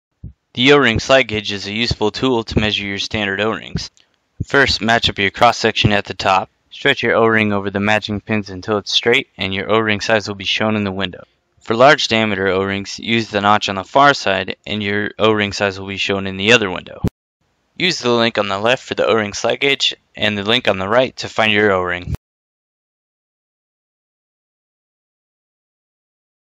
How to Measure an O-Ring - O-Ring Slide Gauge
The O-Ring slide gauge measures AS568 standard O-Rings. It will quickly measure both small and large diameter O-rings. It will measure O-ring sizes: 001-050, 102-178, 201-279, 309-382, and 425-455. Below are links to the slide gauge and related products.